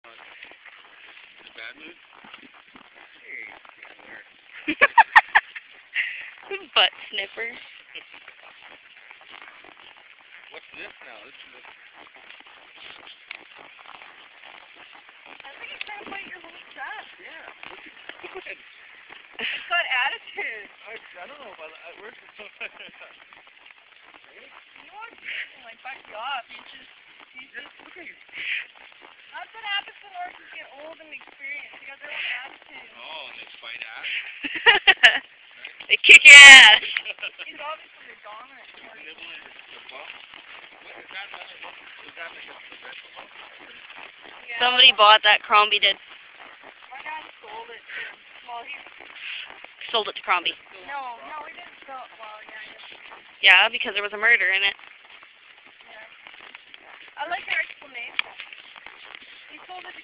Prophet- APHA gelding for sale
"The Jazzmans Prophet" is a beautifully marked 15.1+hh, 2005 Reg. APHA Tobiano gelding who is a very kind, soft and willing boy. "Prophet" has all his ground work in order and is going well both English and Western. "Prophet" has done ring work, is great on the roads & trails and has attended and excelled in both a Polo clinic and a Western Pleasure clinic! "Prophet" has been exposed to cow, llamas, emus and horses, isn't herd bound, has done trot poles and has jumped a few cross rails and did well with no refusals!  "Prophet" has the disposition to excel in anything he is exposed to and could be suitable for an advanced Beginner who knows basic aids! Asking $3500. Interesting trade offers considered. Located in Clearwater, near Kamloops.